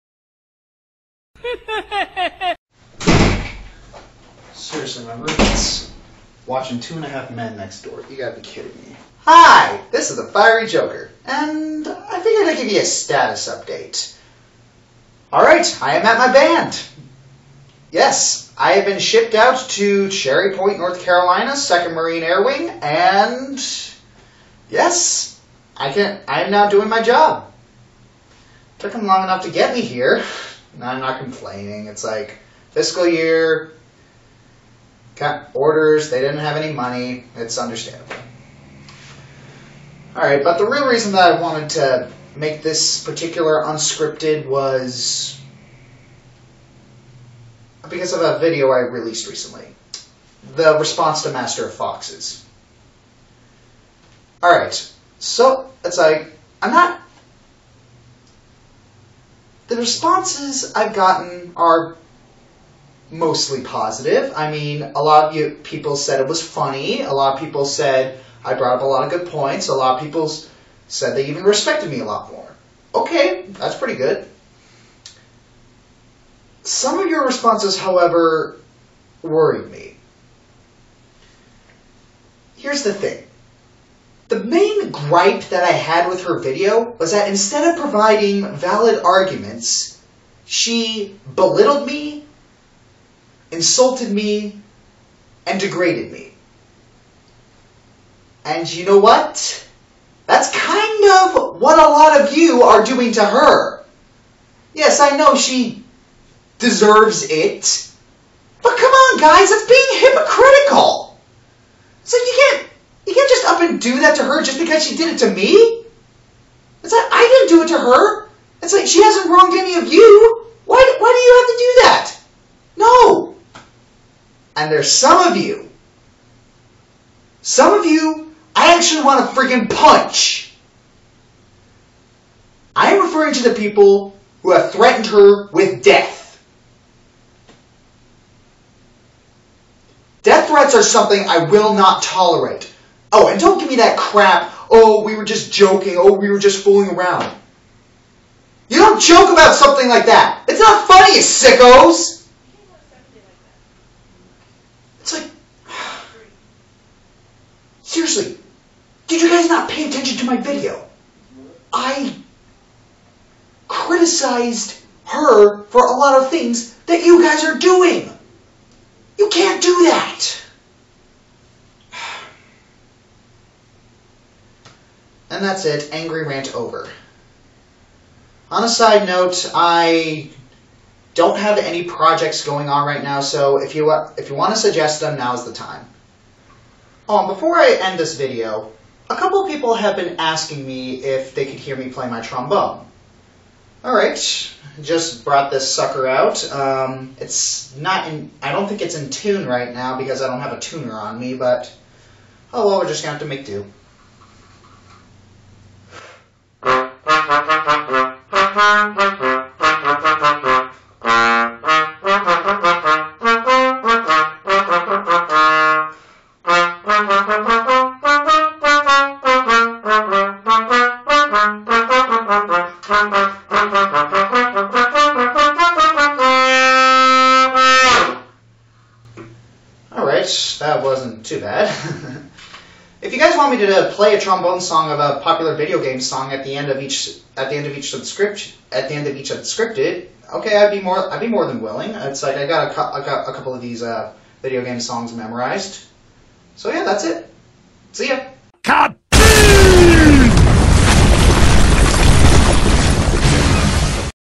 Fiery Joker Unscripted 16: Regarding Master of Foxes
I talk about:
My first duty station
My response to Master of Foxes
Projects
and a little surprise at the end.